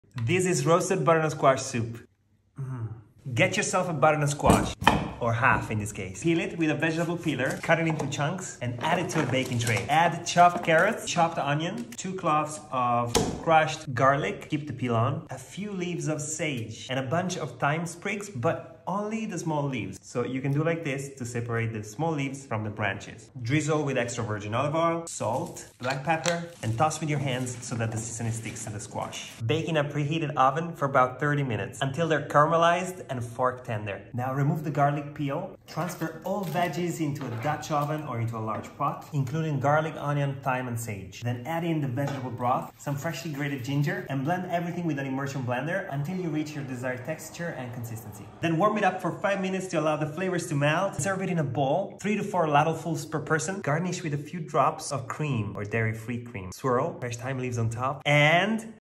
Butternut Squash Soup | Bake-and-Blend for an Easy Soup
⭐️  Making roasted butternut squash soup at home is easy, and the result is much better than the ultra-processed store-bought version.

With our "bake and blend" technique, you can make this recipe with minimal effort and maximum flavor. First, bake the butternut squash with carrot, garlic, onion, and sage until tender and caramelized.

Roasting at high temperatures makes the veggies super tasty and aromatic without you having to cook them on the stovetop.

⭐️ Ingredients
2½ pounds butternut squash peeled, seeded, and diced
½ pound carrots diced
1½ tablespoons extra virgin olive oil
1 onion chopped
2 cloves garlic crushed, with the peel on
5 sprigs thyme only the leaves, no stem
5 leaves sage
2 - 3 cups vegetable stock
1 teaspoon ginger peeled and grated
½ teaspoon salt or more to taste
¼ teaspoon black pepper

⭐️ Bake at 410°F or 210°C for 30 minutes until tender.

Cooked by Nico
Filmed & Edited by Louise
Theplantbasedschool.com